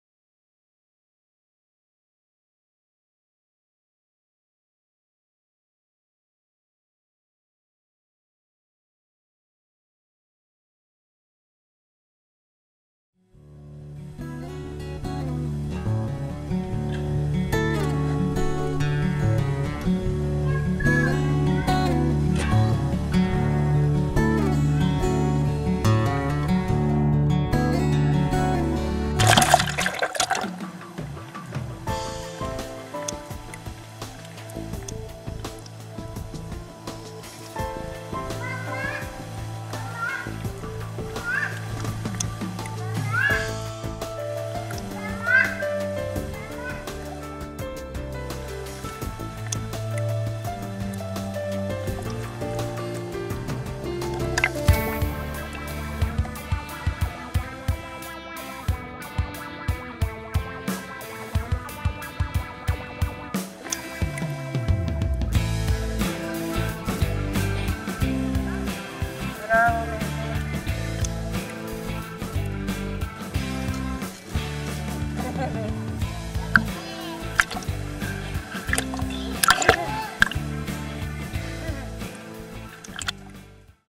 LIAM Surviving english
This video shows what a child can learn to survive or to save itself if accidentally falls in the water.
The silent killer, as it is called: drowning is one of the leading causes of accidental death in children under 5 years of age.
Constant direct supervision is the best.  But accidents happened in seconds of distraction.  In 86% of drownings children were fully clothed.
If all the precautions fail (barriers, alarms, supervision. ) Give your child the gift of life by teaching him to roll over and float to breathe, cry for help, and wait for rescue. He will have a chance to survive.
I takes 4 weeks to learn.
Guatemala.